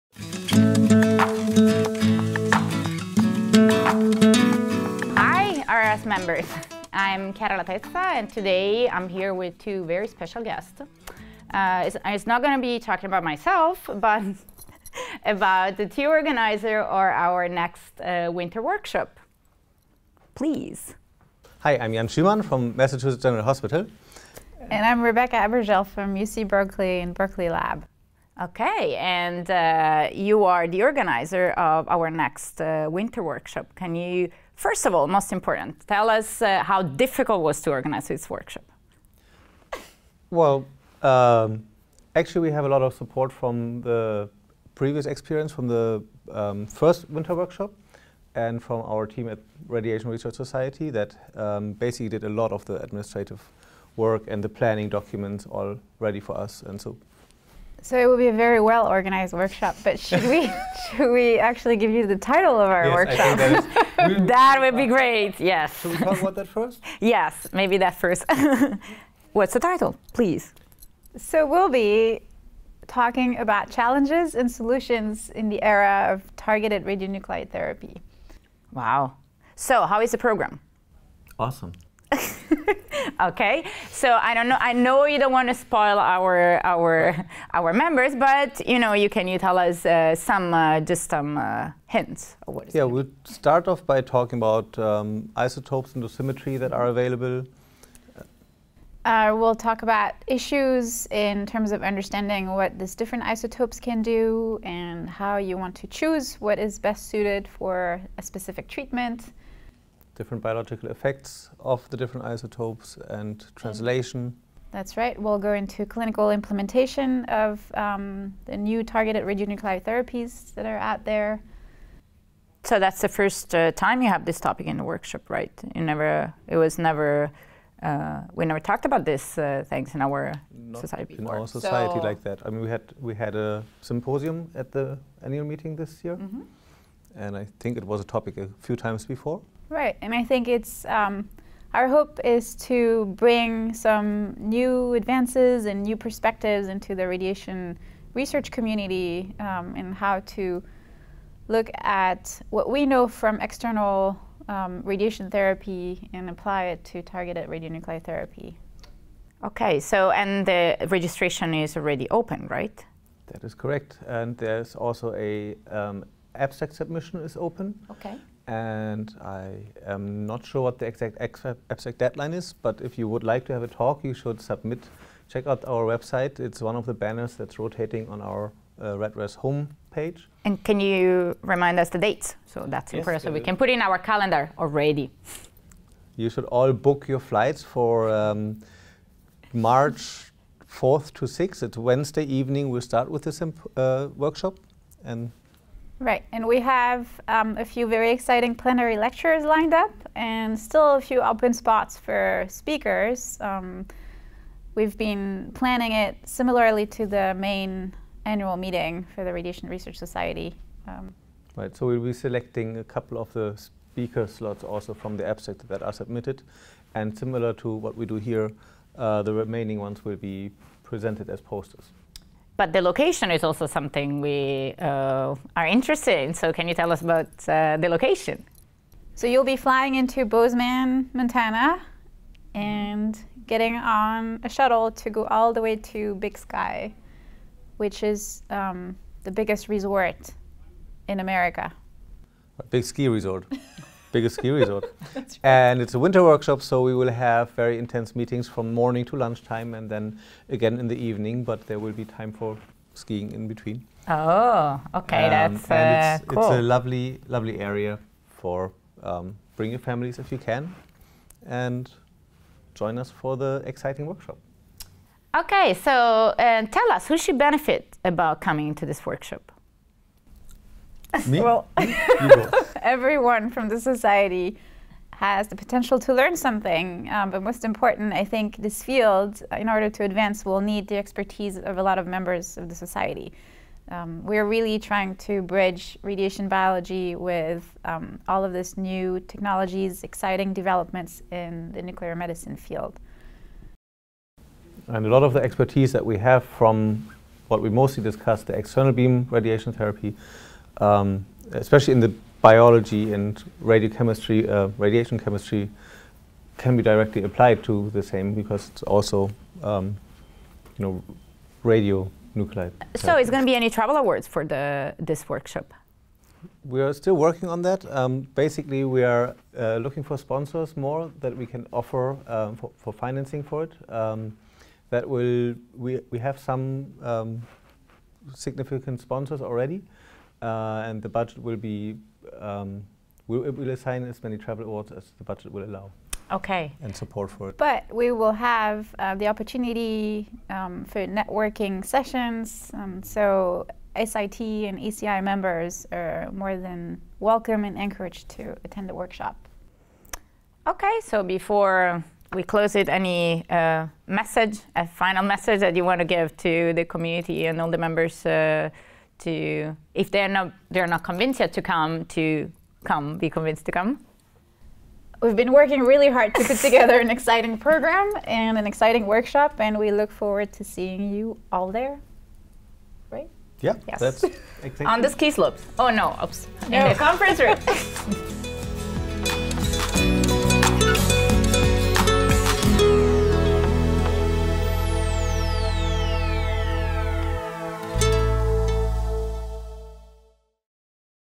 2020 Winter Workshop: Challenges and Solutions in the Era of Targeted Radionuclide-Based Therapy
Dr. Chiara La Tessa sits down with Drs. Jan Schuemann and Rebecca Abergel to discuss the 2nd RRS Winter Workshop, "Challenges and Solutions in the Era of Targeted Radionuclide-Based Therapy." Schuemann and Abergel, who are co-chairing the workshop, detail the program emphasis & who can benefit from attending. Submit an abstract and be considered to speak! Learn more, view the call for abstracts and register